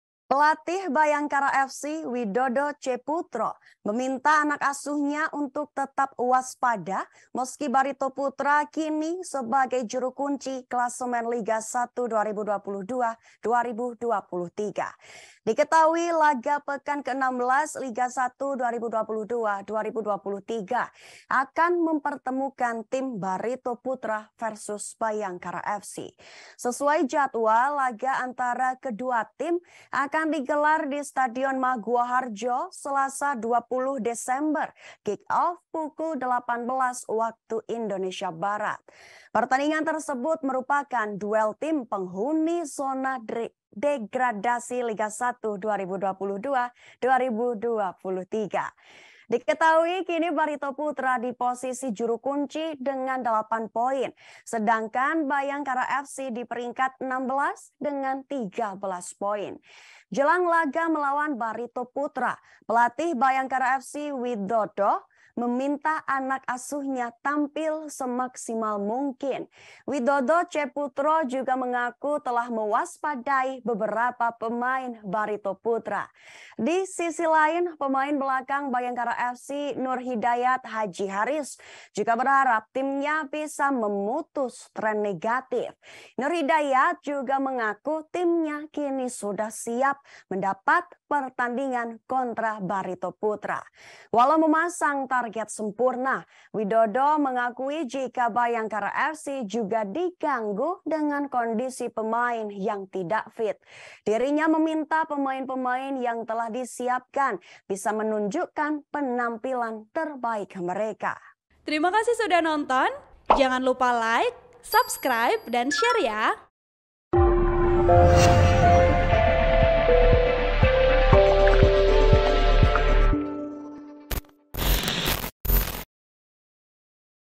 Bhayangkara FC Vs Barito Putera di Pekan 16 Liga 1 2022/2023, Pelatih Ingin Anak Asuh Tetap Waspada
TRIBUN-VIDEO.COM - Pelatih Bhayangkara FC, Widodo C Putro meminta anak asuhnya untuk tetap waspada meski Barito Putera kini sebagai juru kunci klasemen Liga 1 2022/2023.

Diketahui, laga pekan ke-16 Liga 1 2022/2023, akan mempertemukan tim Barito Putera vs Bhayangkara FC.

Sesuai jadwal, laga antara Barito vs Bhayangkara FC akan digelar di Stadion Maguwoharjo, Selasa (20/12) kick off pukul 18.00 WIB.

Pertandingan tersebut merupakan duel tim penghuni zona degradasi Liga 1 2022/2023.

Diketahui, kini Barito Putera di posisi juru kunci dengan delapan poin,sedangkan Bhayangkara FC di peringkat ke-16 dengan 13 poin.

Jelang laga melawa Barito Putera, pelatih Bhayangkara FC, Widodo C Putro meminta anak asuhnya tampil semaksimal mungkin.

Widodo C Putro juga mengaku telah mewaspadai beberapa pemain Barito Putera.

Di sisi lain, pemain belakang Bhayangkara FC, Nurhidayat Haji Haris juga berharap timnya bisa memutus tren negatif.

Nurhidayat juga mengaku timnya kini sudah siap menapat pertandingan kontra Barito Putera.

Walau memasang taget sempurna, Widodo mengakui jika Bhayangkara FC juga diganggu dengan kondisi pemain yang tidak fit.

Dirinya meminta pemain-pemain yang telah disiapkannya, bisa menunjukan penampilan terbaik.

(Tribun-Video.com)

Artikel ini telah tayang di Tribunnews.com dengan judul Bhayangkara FC vs Barito Putera Liga 1, Coach Widodo Minta Anak Asuhnya Tak Remehkan Lawan,
Penulis: Hafidh Rizky Pratama
Editor: Siti Nurjannah Wulandari